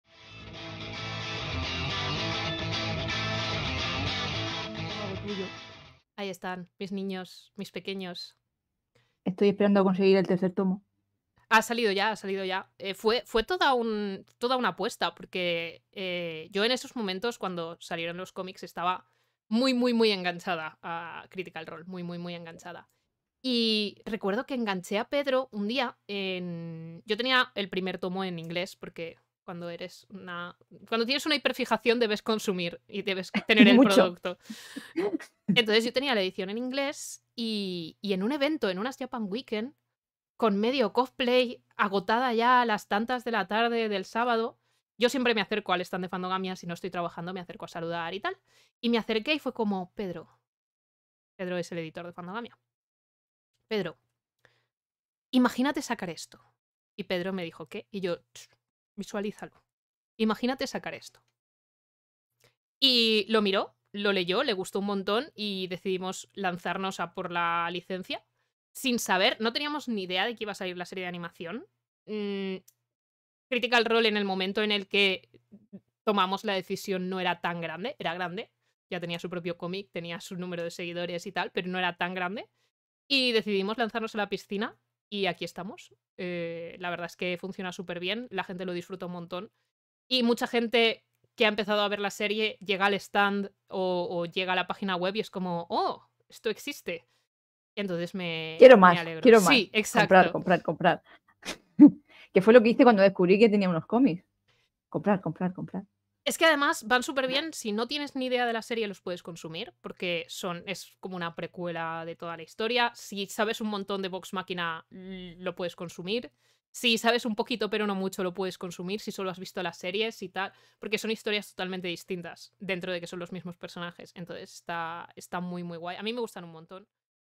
OASIS Entrevista a Liza, sobre Vox Machina
En este fragmento Liza nos habla sobre su afición a Critical Role y la traducción de Vox Machina